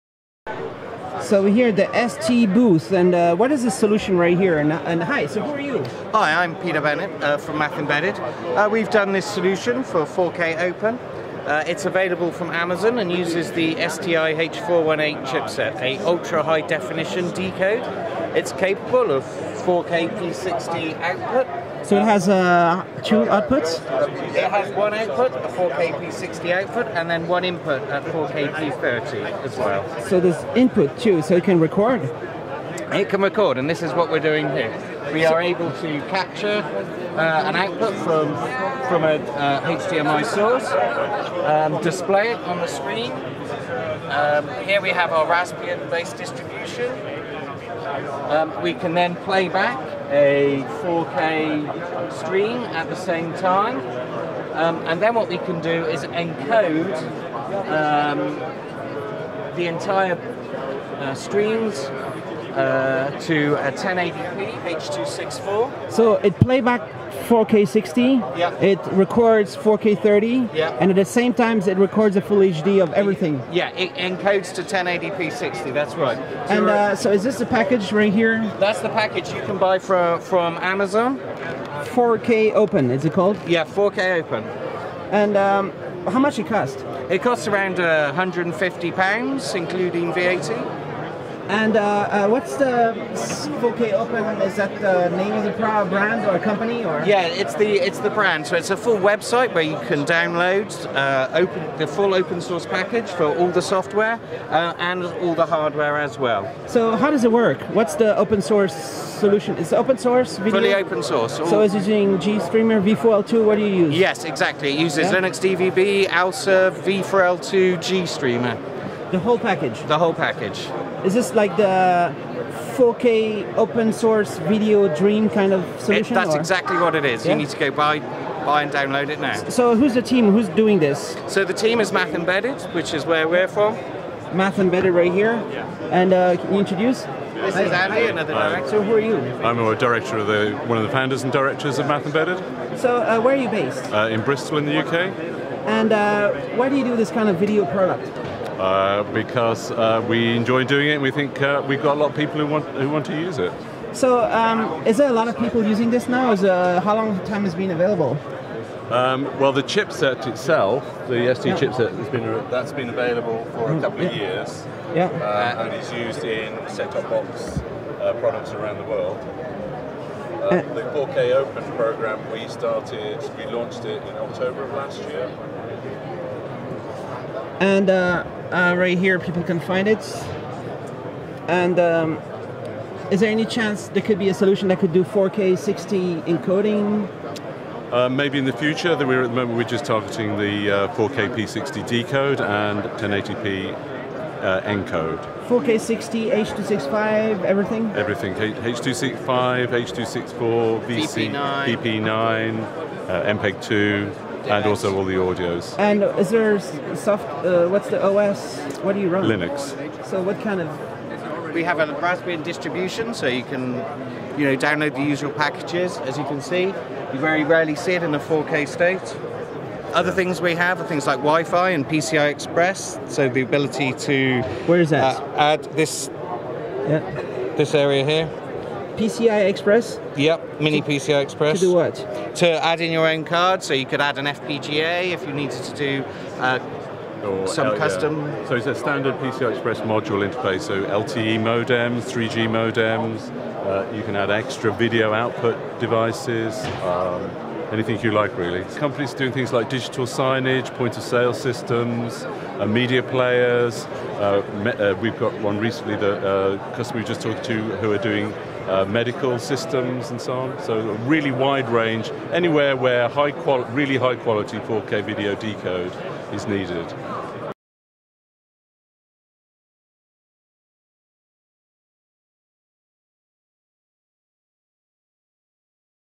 Open source 4K video platform (STmicroelectronics, MathEmbedded, Charbax)
Charbax interviewed MathEmbedded about their 4K open platform!